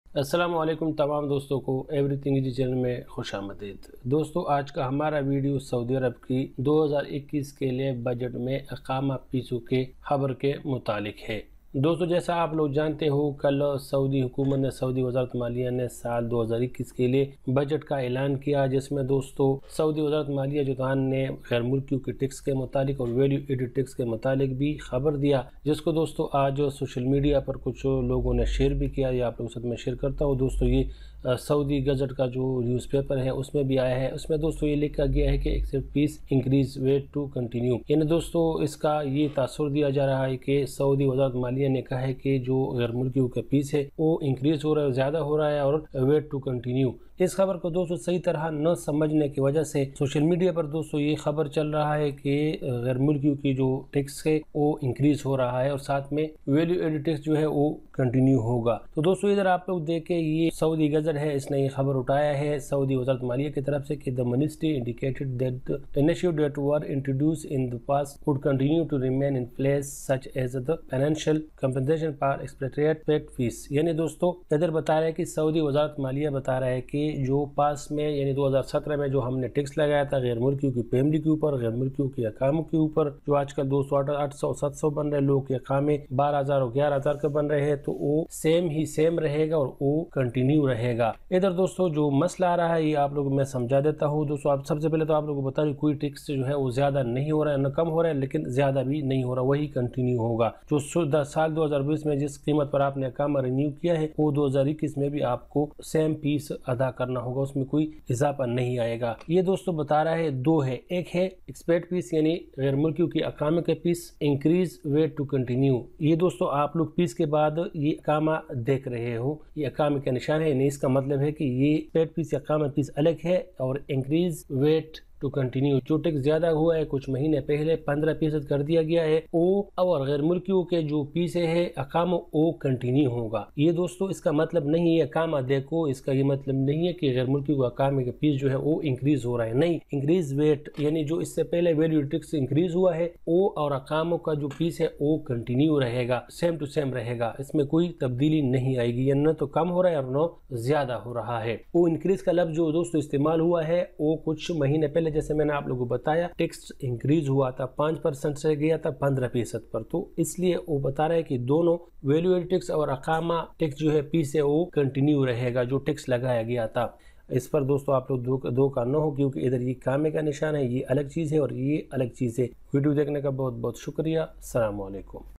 saudi ministry of finance about expatriates expat fee || 2021 budget || every thing easy
saudi ministry of finance about expatriates (expat fee), in 2021 budget every thing easy